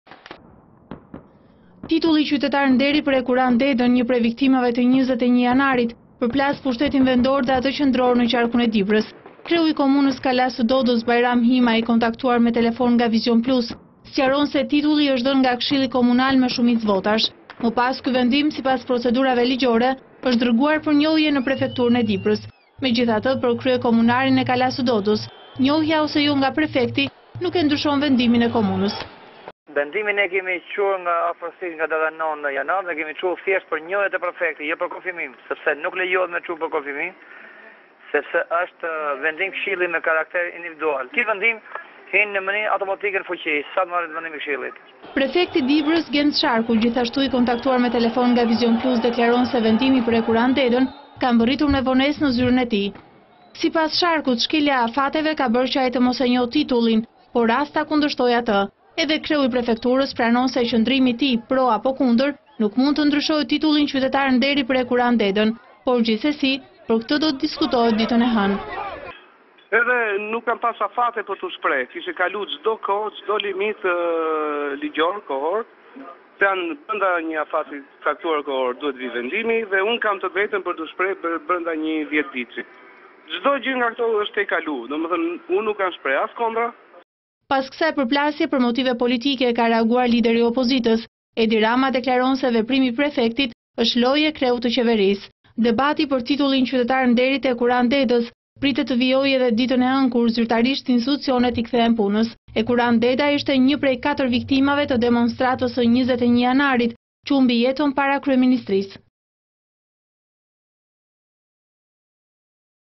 Përplasje për titullin e Hekuran Dedës - Vizon Plus - News - Lajme
Titulli Qytetar Nderi për Hekuran Dedën, një prej viktimave te 21 Janarit, përplas pushtetin vendor me atë qendror ne qarkun e Dibrës. Kreu i komunës se Kalasë se Dodes Bajram Hima, i kontaktuar me telefon nga "Vizion Plus", sqaron se titulli i është dhënë nga këshilli komunal me shumice votash. Me pas, ky vendim, sipas procedurave ligjore, është dërguar për njohje ne prefekturën e Dibrës. Megjithatë, për kryekomunarin e Kalasë se Dodes njohja ose jo nga prefekti nuk e ndryshon vendimin e komunës. Prefekti i Dibrës Genc Sharku gjithashtu i kontaktuar me telefon nga Vizion Plus deklaron se vendimi për Hekuran Dedën ka mbërritur me vonese ne zyrën e tij. Sipas tij, shkelja e afateve ka bere qe ai te mos e njohe titullin, por as te mos e kundërshtojë atë. Edhe kreu i prefekturës pranon se qëndrimi i tij pro apo kundër nuk mund te ndryshoje titullin Qytetar Nderi për Hekuran Dedën. Por gjithsesi për këtë do te diskutohet te hënën. Pas kësaj përplasje me motive politike ka reaguar lideri i opozitës. Edi Rama deklaron se veprimi i prefektit loje e kreut te qeverise. Debati për titullin qytetar te Hekuran Dedës pritet te vijoje edhe ditën e hënë kur zyrtarisht institucionet i kthehen punës. Hekuran Deda ishte një prej 4 viktimave te demonstratës se 21 janarit qe humbi jetën para Kryeministrisë.

Title of Honorary Citizen of Hekuran Deda, one of the victims of January 21, collided with the central government power in the district of Dibra. The head of the municipality that castle that Dodes Bajram Hima, contacted by telephone by "Vision Plus", explains that the title given by the municipal council by majority vote. Later, this decision, according to legal procedures, is sent for recognition in the prefecture of Debar. However, the castle that Dodes communal recognition by the mayor or not does not change the decision of the municipality. Prefect of Debar Genc Sharku also contacted by telephone by Vision Plus declares that the decision to Hekuran Deda has arrived late at his office. According to him, the violation of terms has led him to not recognize the title, but not even challenge it. The head of the prefecture recognizes that his position for or against can not change the title of Honorary Citizen of Hekuran Deda. But anyway this will be discussed on Monday. After this confrontation with the political motives of the opposition leader has reacted. Edi Rama, the mayor declared that the action of playing the head of government. Debate on the title Deda Hekuran citizen is expected to continue even on Monday when the institutions officially back to work. Hekuran Deda was one of four demonstration that the 21 victims who died before January the Prime Minister.